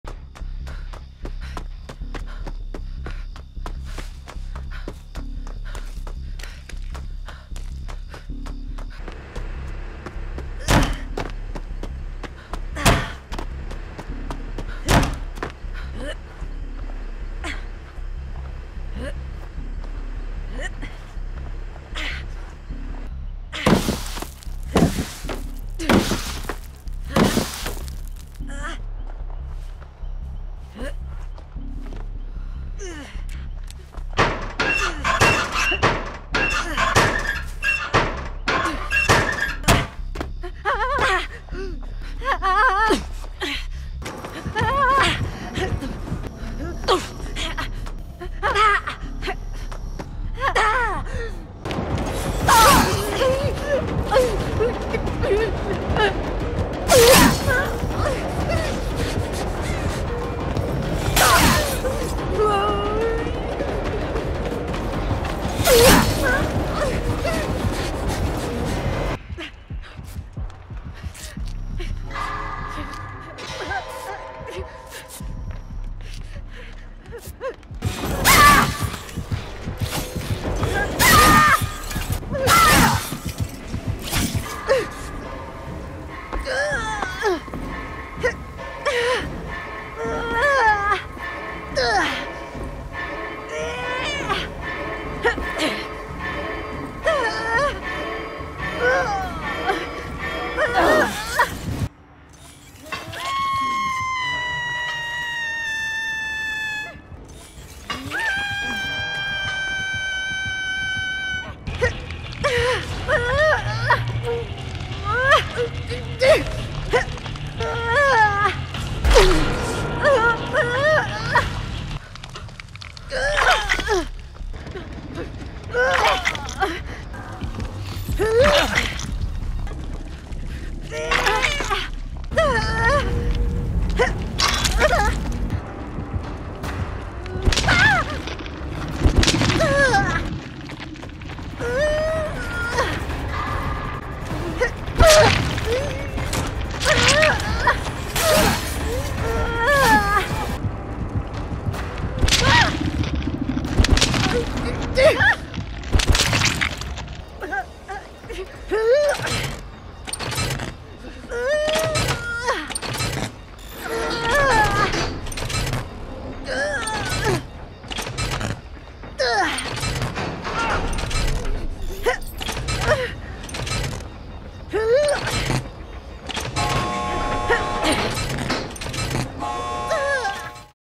Thalita Lyra Voice
In this video I will be showcasing the voice of Thalita Lyra during in game. This video will not be showcasing the voice interactions she has with her brother, Renato Lyra instead it'll show off her voice SFX when vaulting a pallet, window, getting hooked, getting injured etc...
0:00 - 0:08 Breathing Sounds
0:09 - 0:22 Window Vaulting
0:23 - 0:33 Pallet Vaulting
0:34 - 0:39 Locker SFX
0:40 - 0:51 Falling Sounds
0:52 - 1:09 Getting Hit
1:10 - 1:16 Injured Sounds
1:17 - 1:23 Going Down
1:24 - 1:40 Wiggling SFX
1:41 - 1:50 Getting Hooked
1:51 - 2:01 Attempting To Escape Hook Sounds
2:01 - 2:11 Unhooking Survivors
2:12 - 2:14 4%
2:15 - 2:29 2nd Stage & Struggling On Hook
2:30 - 2:36 Death Hook
2:37 - 3:00 Opening Exit Gate SFX